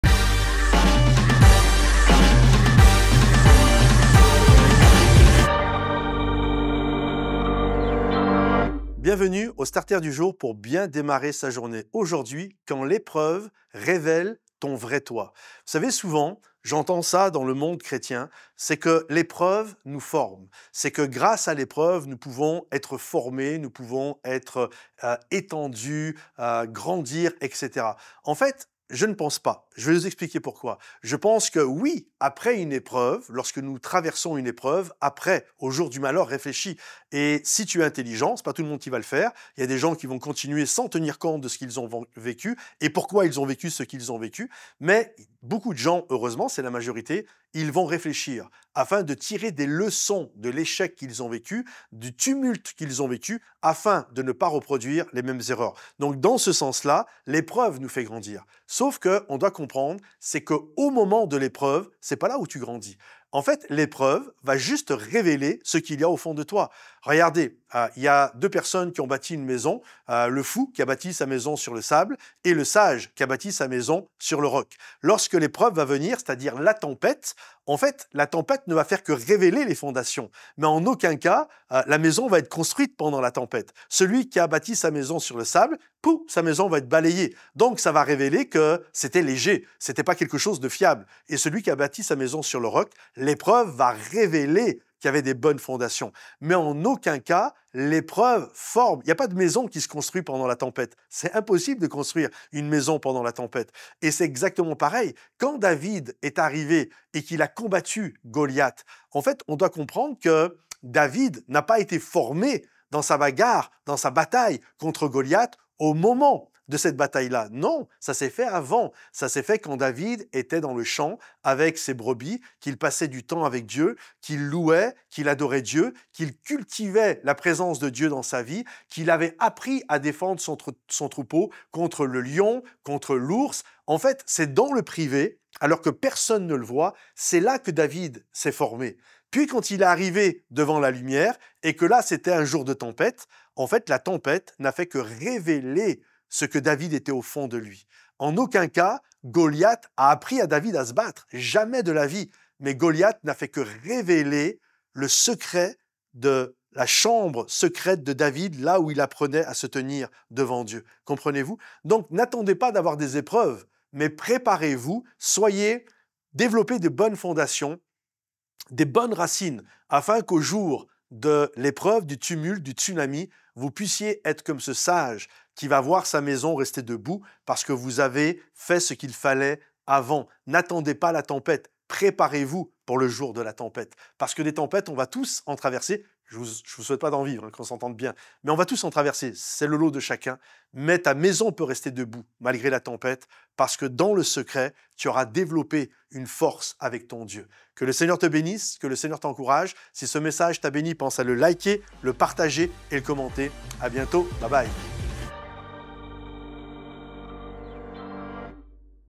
STARTER - Quand l'épreuve révèle ton vrai toi
Nous entendons souvent que l'épreuve nous forme. Mais… ce n'est pas exactement vrai. Aujourd'hui, découvre ce que produit réellement l'épreuve en toi, et comment véritablement grandir.

Restons connectés !